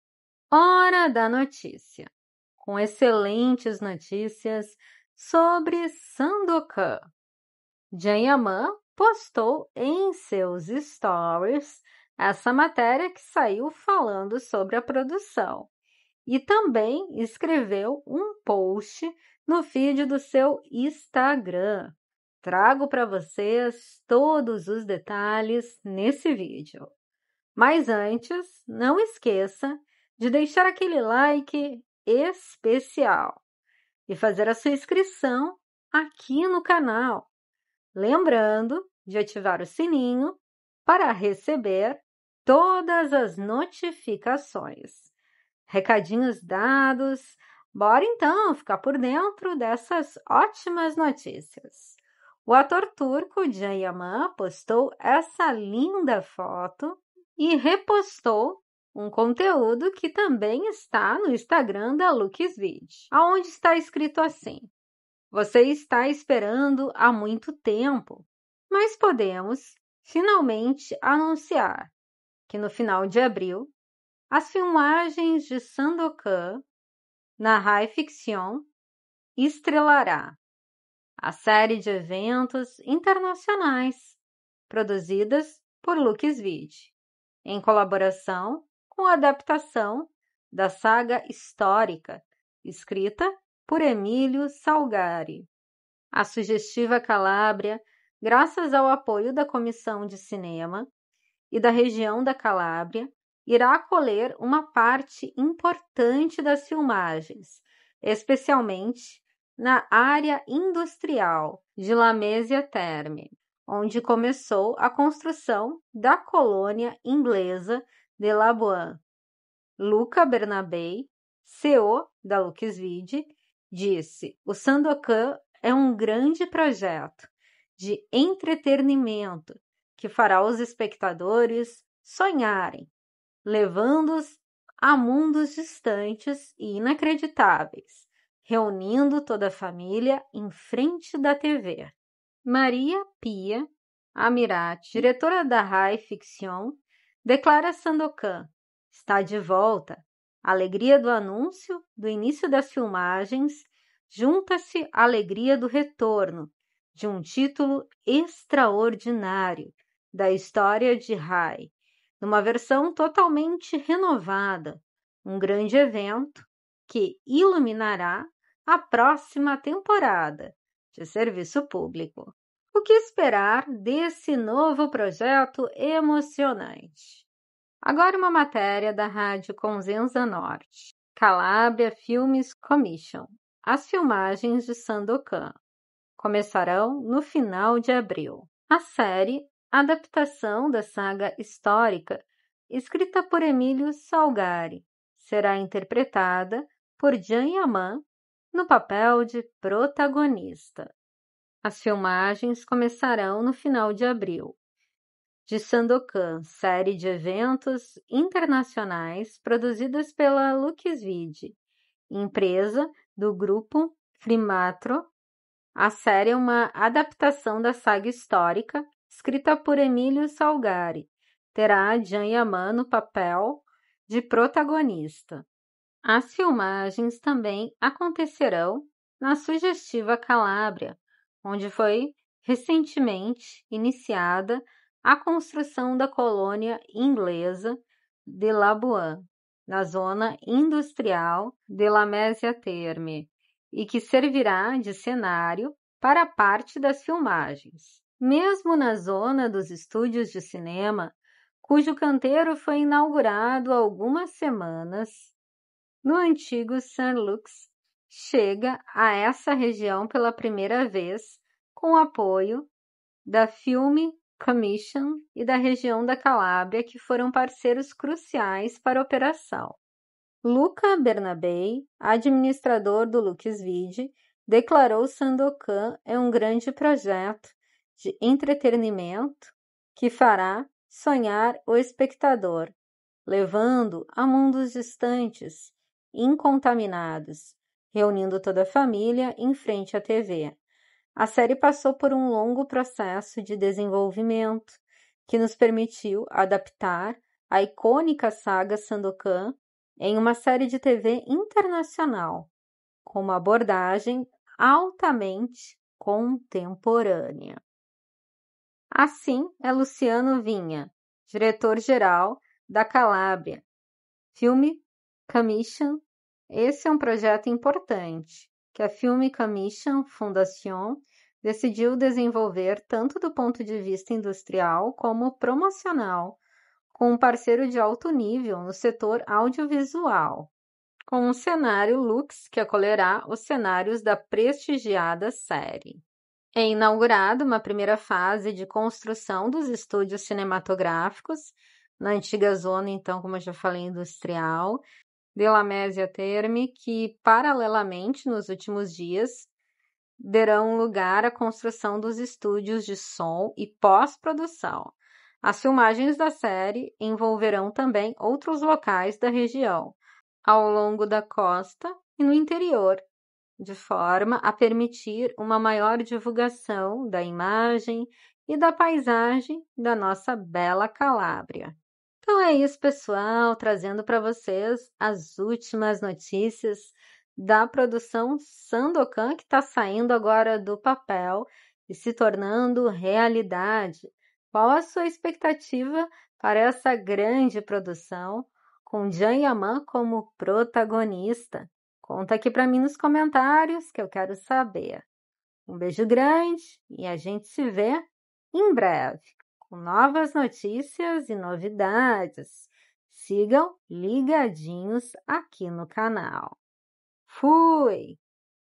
⚠️💥🗣🗞 NOTÍCIAS SOBRE CAN YAMAN EM SUA NOVA PRODUÇÃO SANDOKAN
Vídeo com alguns detalhes de Sandokan

Saiba das últimas notícias e atualizações ligadas ao ator turco Can YamanDemet Özdemir, Özge Gürel.